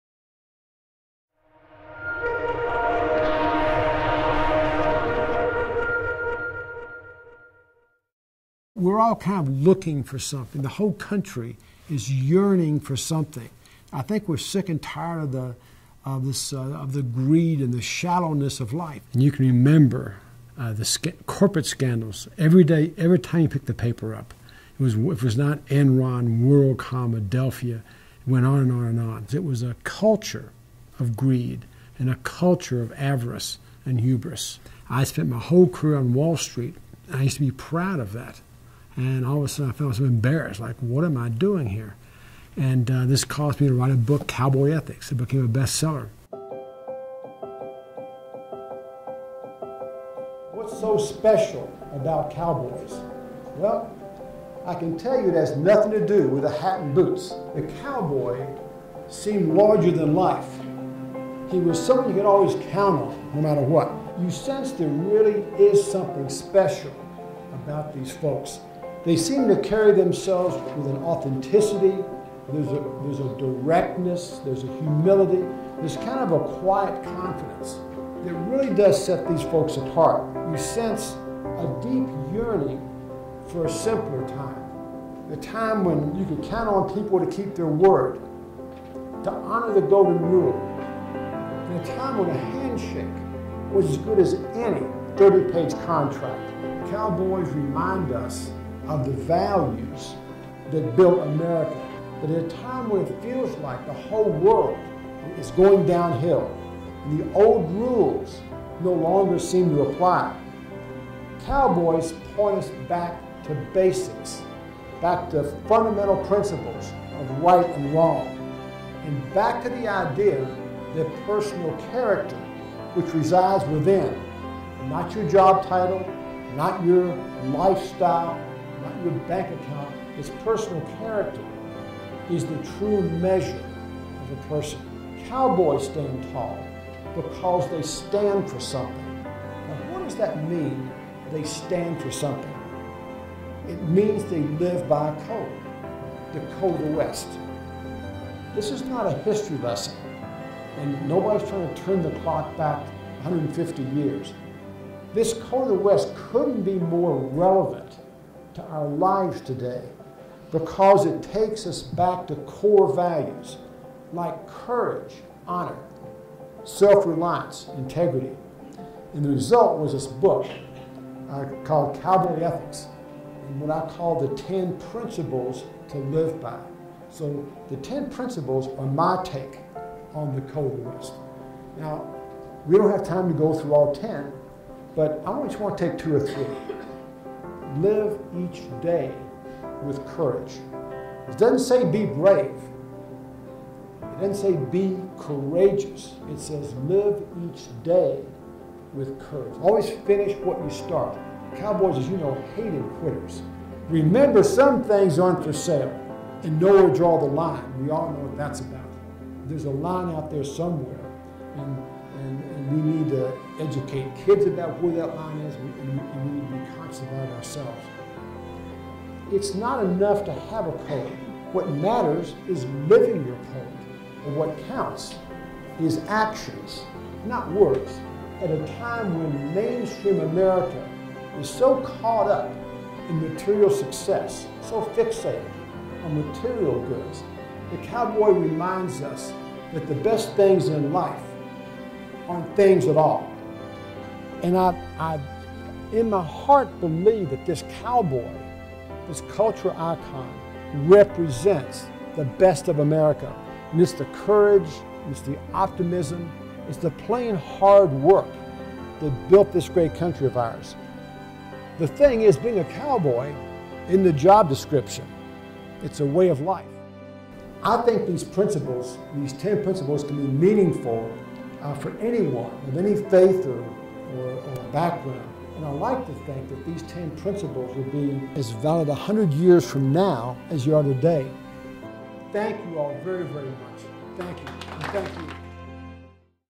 James P Owen: Cowboy Ethics
Best selling author James P Owen speaking at the Center about his book Cowboy Ethics, the code of the west, business ethics and how modern society can benefit from these uniquely American values.